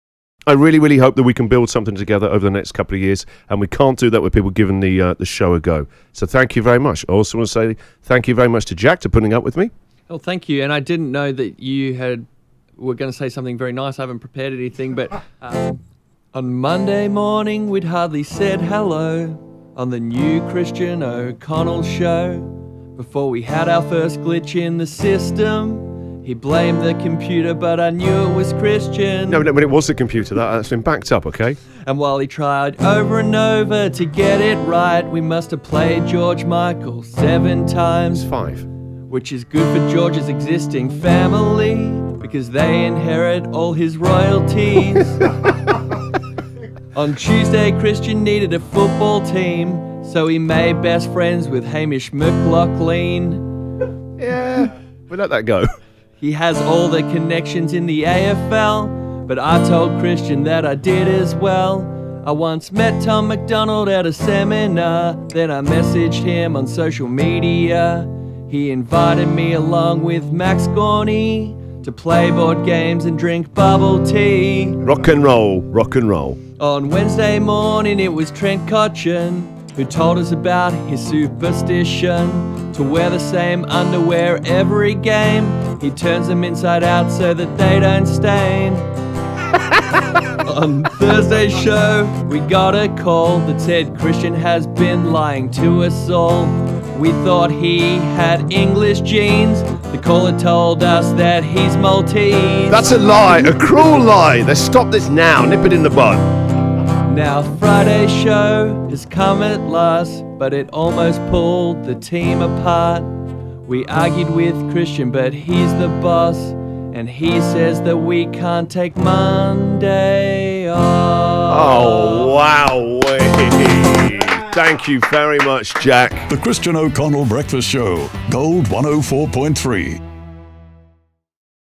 First week of Christian O'Connell in song form
Jack has written a beauty of a song to summarize the week!